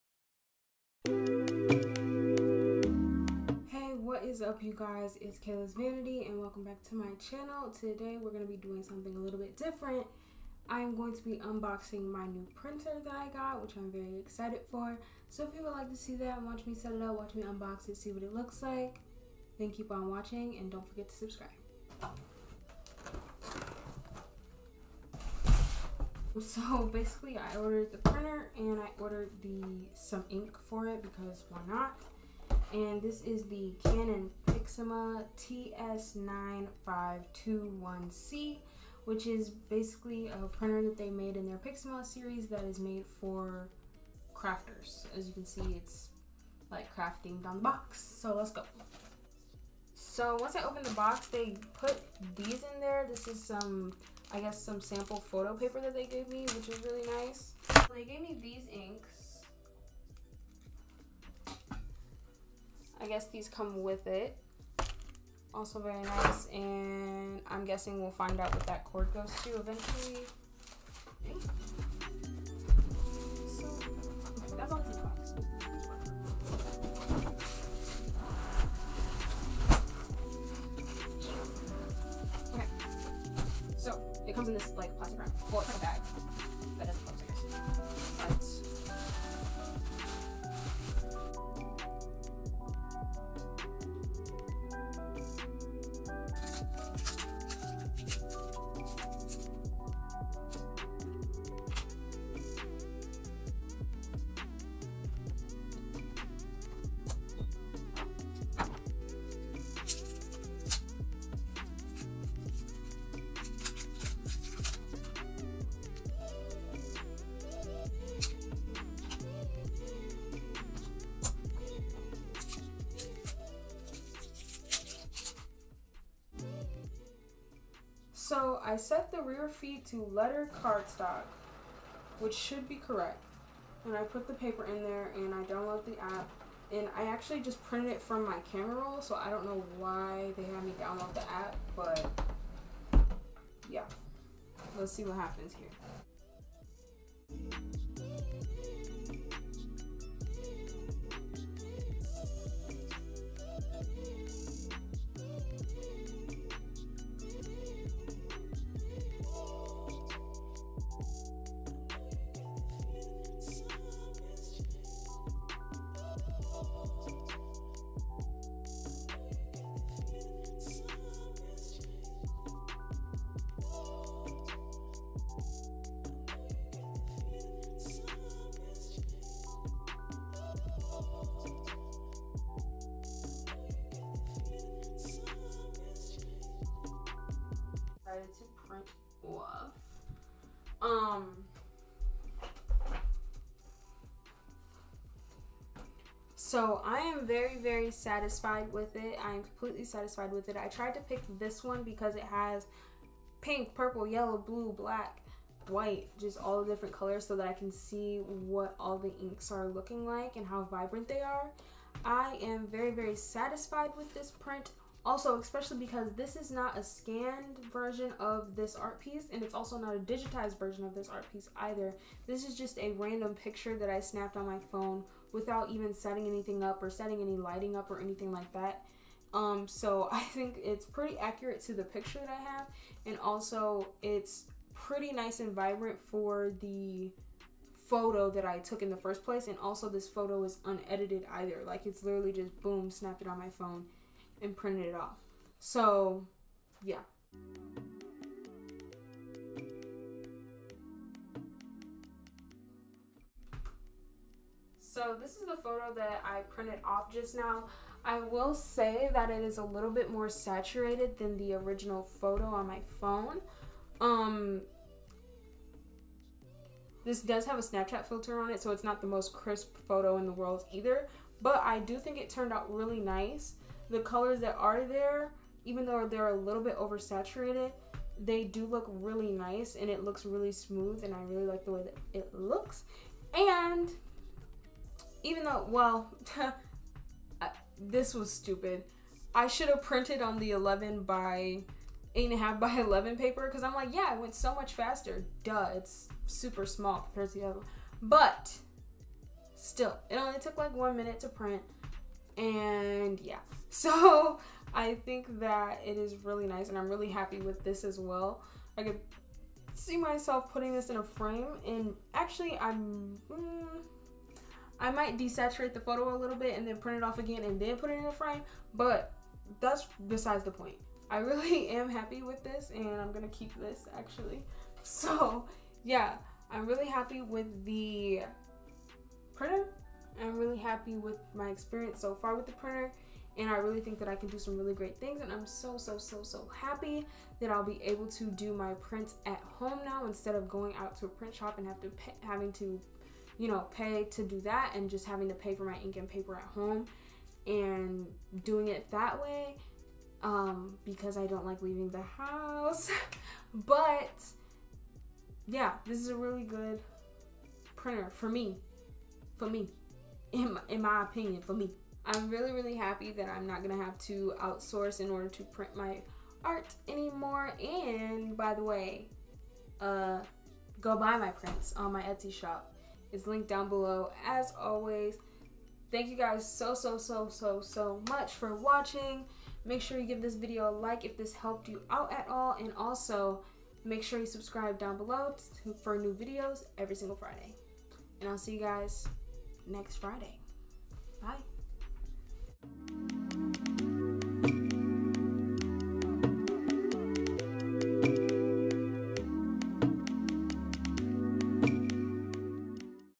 UNBOXING AND TRYING OUT MY NEW CANON PRINTER | KAILAS VANITY
Hey guys, thank you all for tuning in with me this week to unbox and try out my new Canon TS9521C Crafting and Photo Printer.